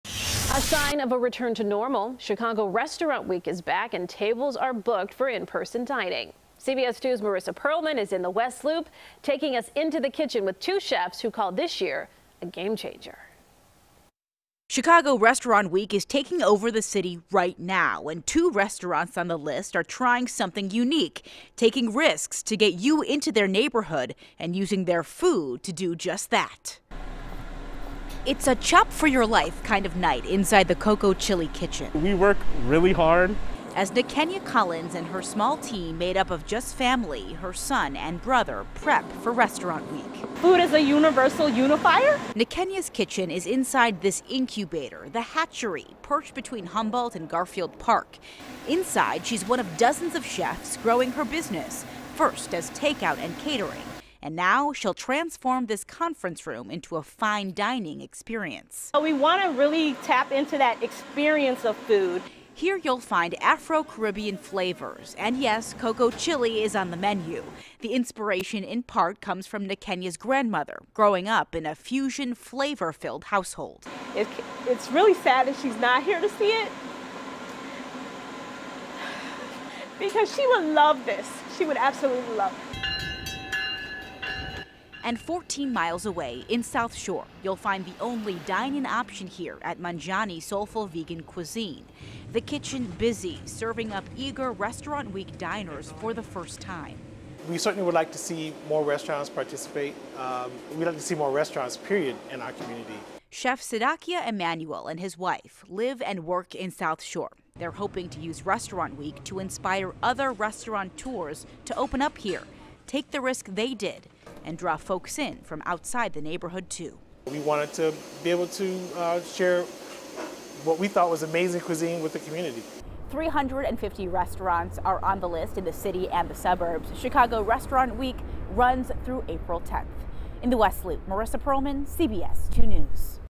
Two chefs call this year's Chicago Restaurant Week a game-changer
The chefs are trying something new in hopes of bringing you into their neighborhood – and they’re using their food to just that. CBS 2's Marissa Perlman reports.